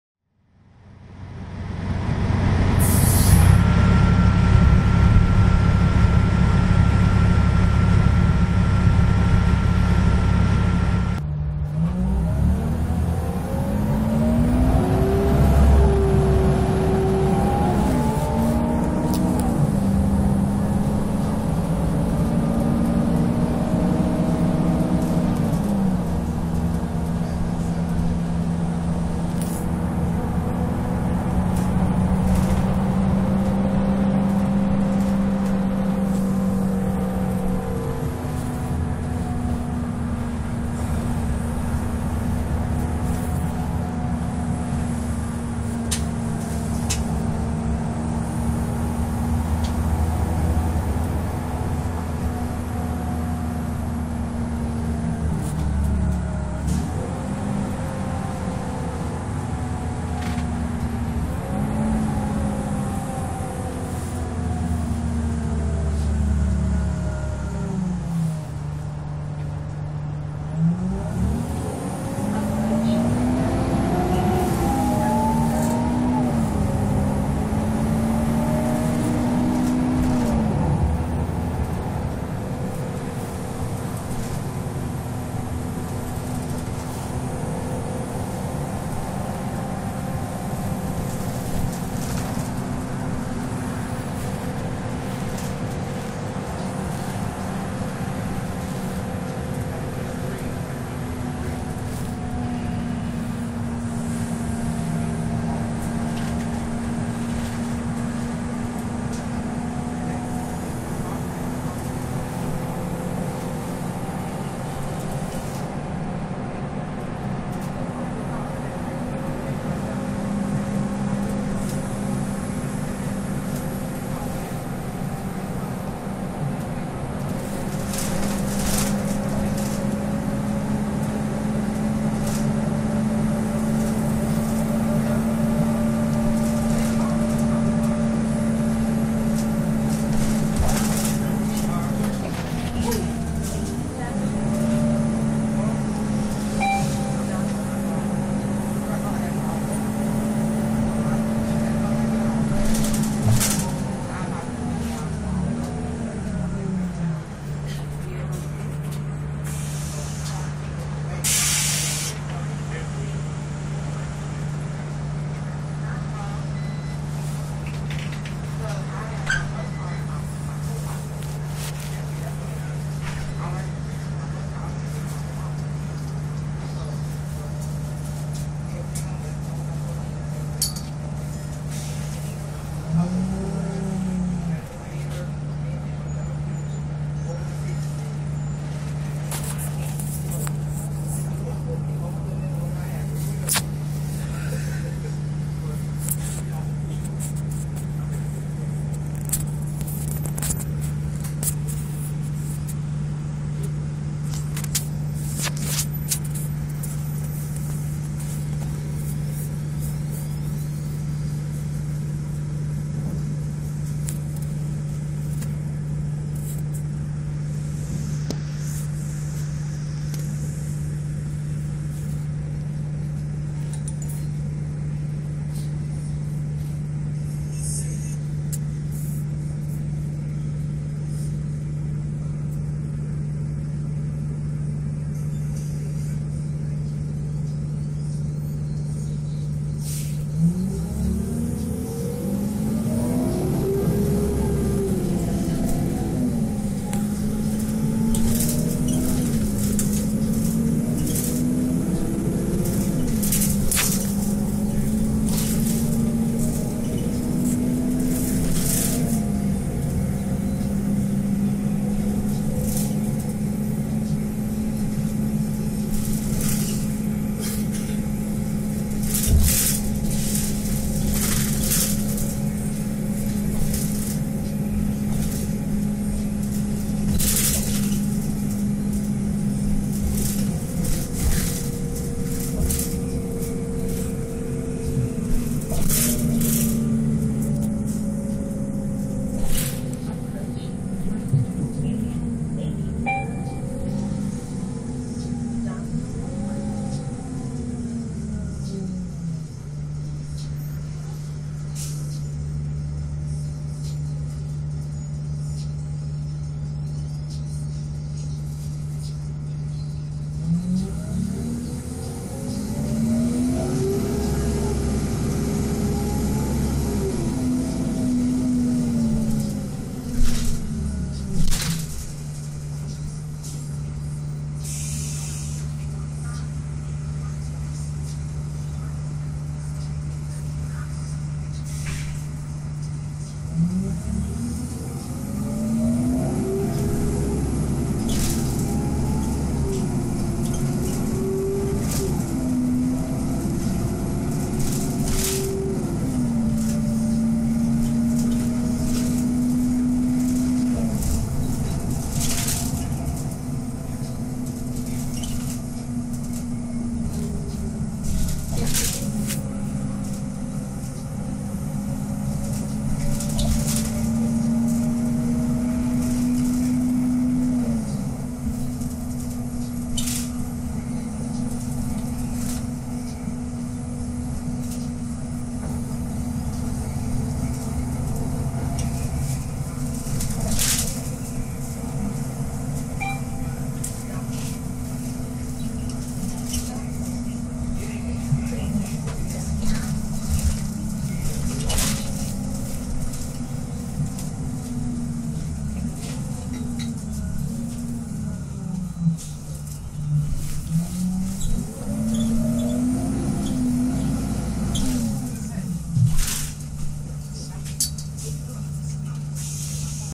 Pace NABI D40LFW #6208 - 349 South Western
Here’s a throwback of Pace Nabi #6208 running northbound to 79th/Western on the 349. This bus is equipped with a DDS50 EGR with an Allison World Tranny.